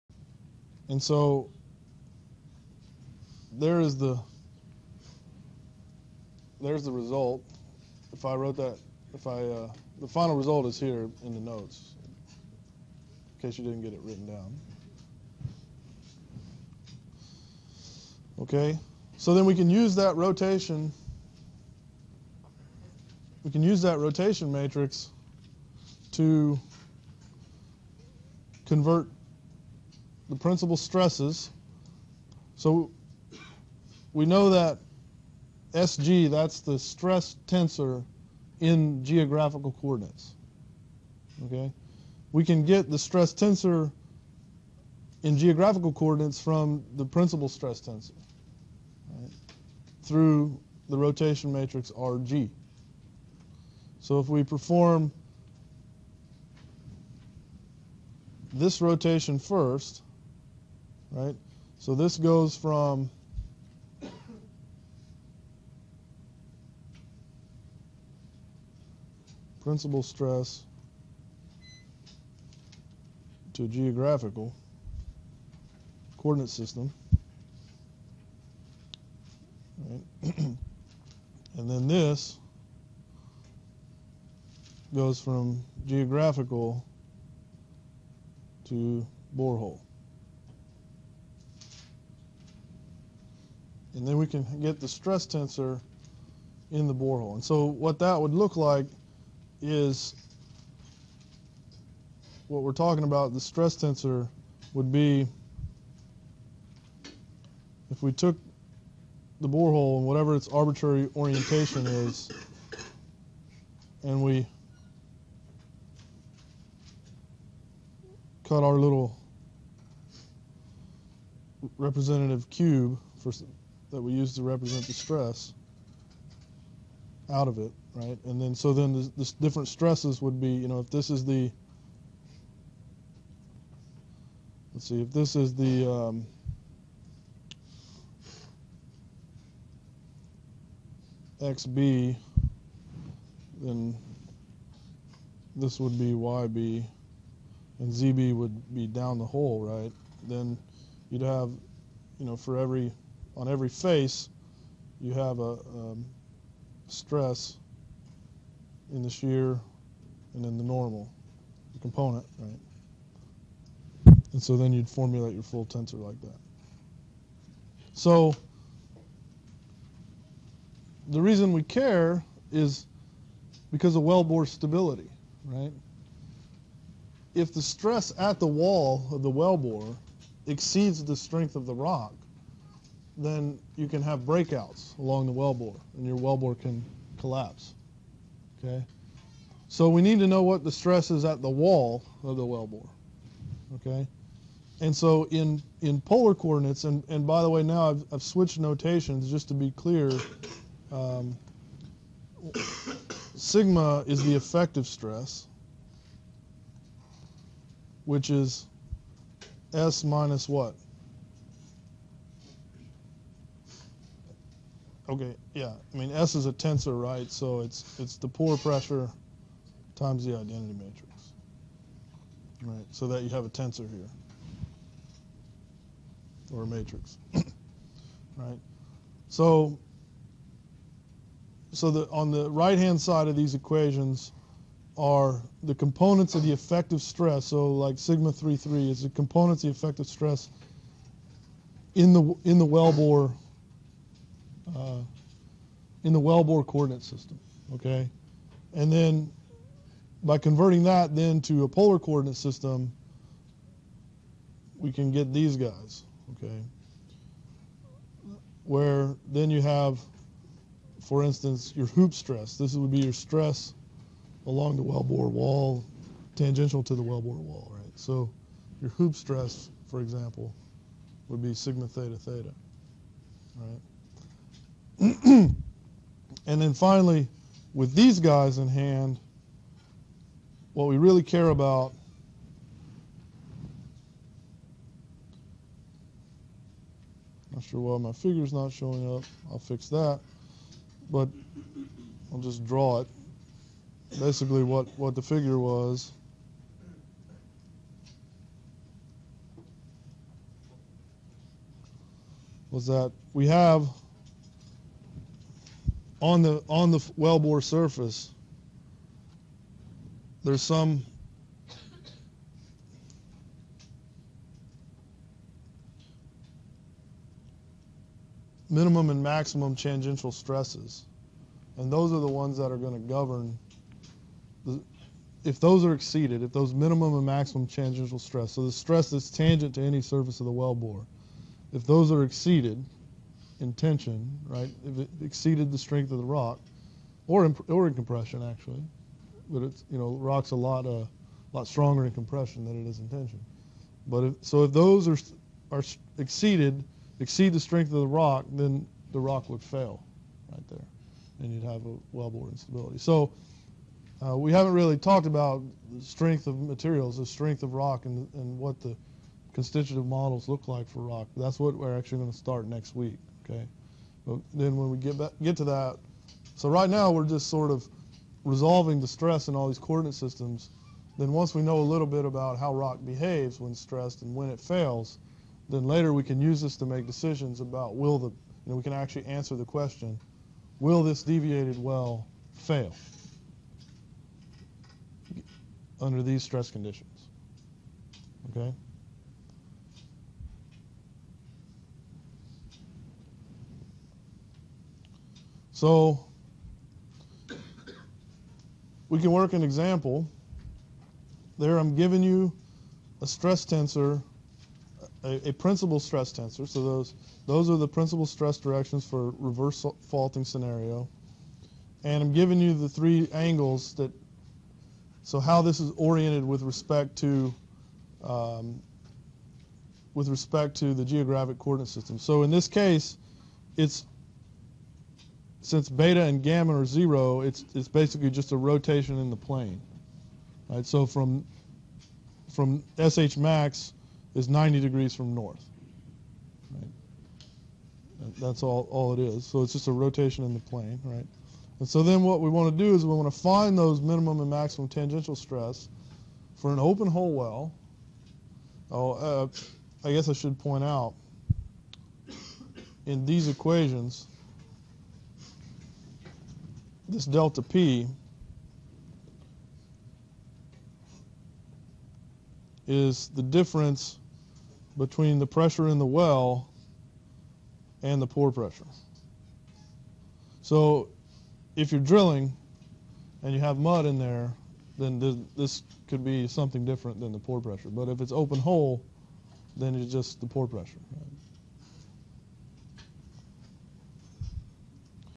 Wellbore stress, Reservoir Geomechanics (Geology)
Stresses in the wellbore coordinate system.

Principal effective stresses around the wellbore

Reverse faulting